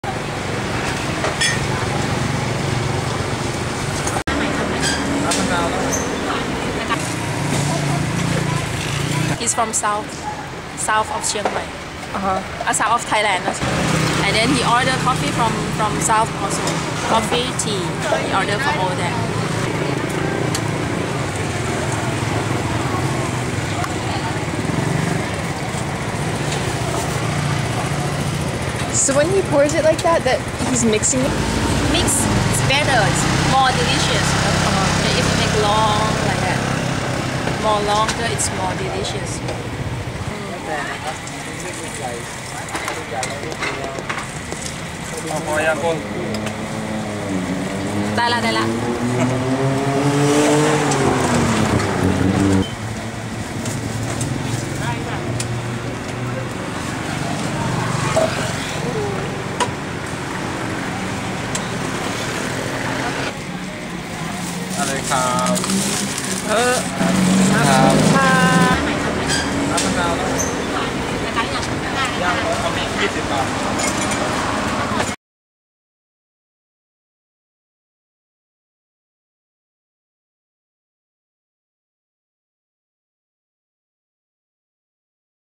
Making Famous Thai Coffee
The Thai love their coffee! There's outdoor coffee stands in the morning, brewing coffee with cans of carnation milk stocked up for sweetening. Here's a coffee stand I found outside the Chiang Mai walking streets and the maker was mixing them in a special way. Watch the video to find out why...

Travel Survival | Solo Travel | Live with GRRR!